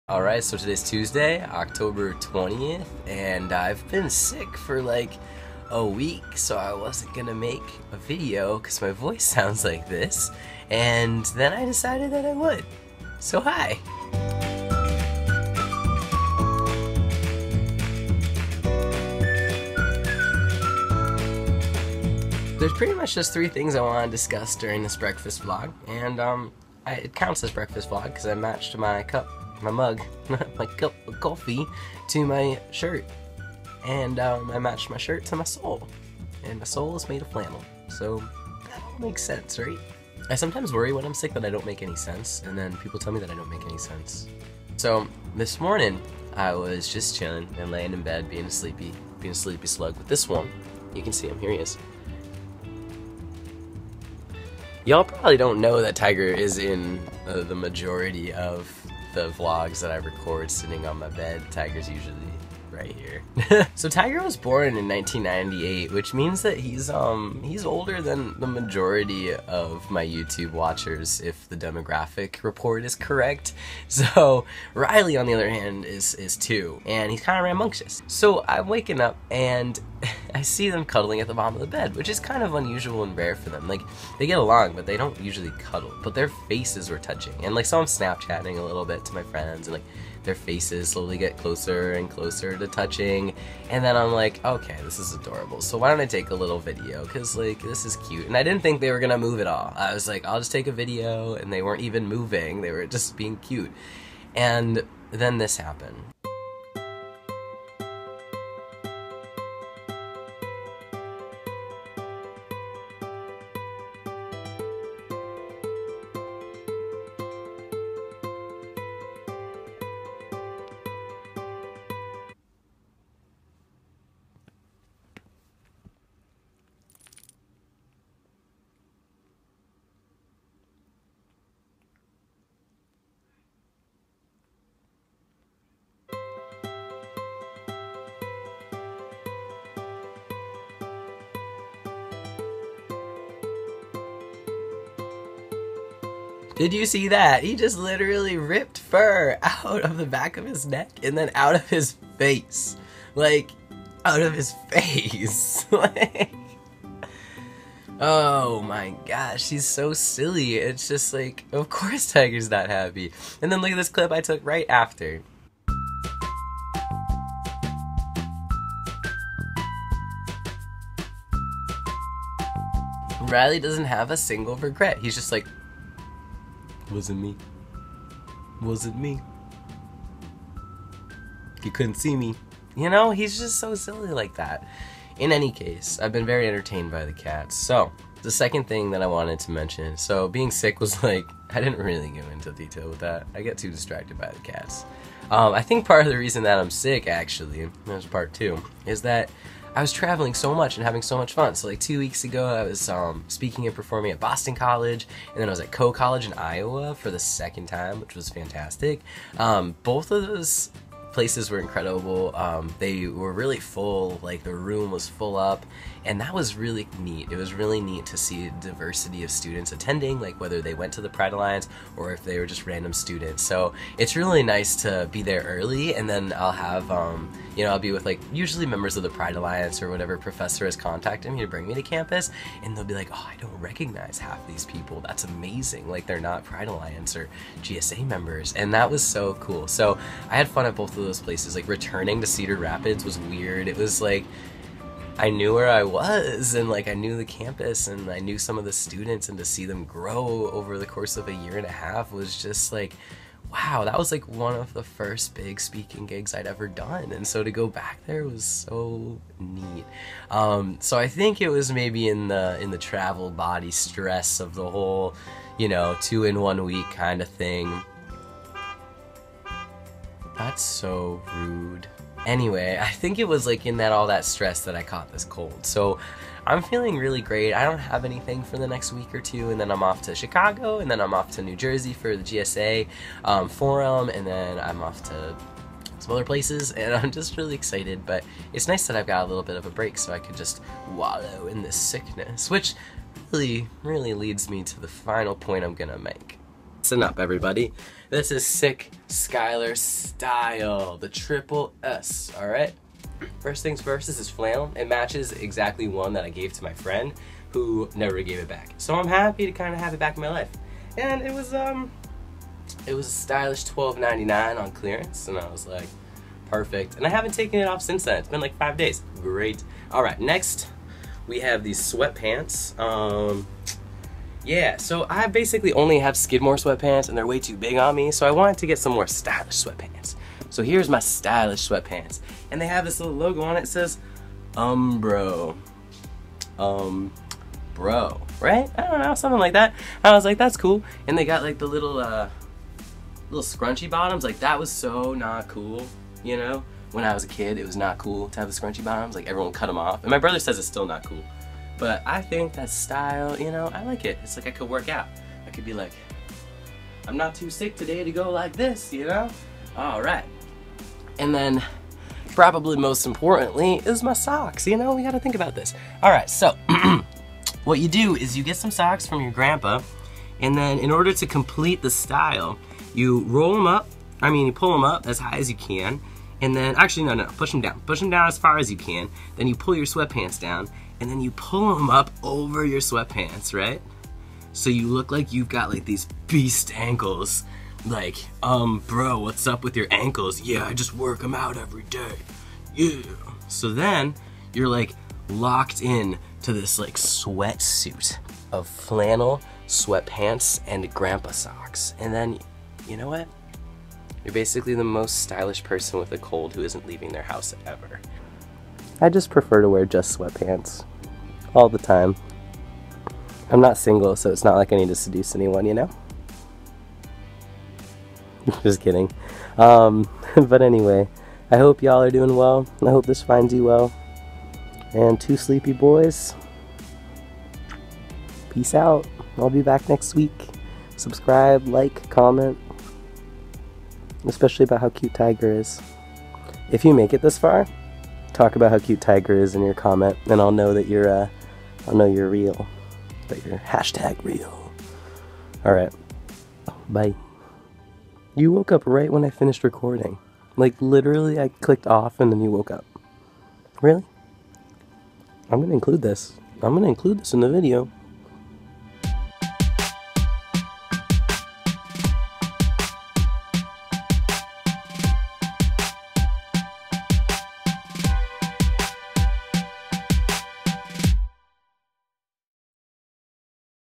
BREAKFAST VLOG: CATS & SILLY OUTFIT OF THE DAY
Some transgender related speaking gigs I've done, my cats, being sick, and a jokingly rendered OOTD ;) Jump links and info below!

Jump links:
Cat attack - 1:45
Being sick - 3:17
OOTD - 4:56
Outro - 6:48

Less human, more being. Keep being. I love you.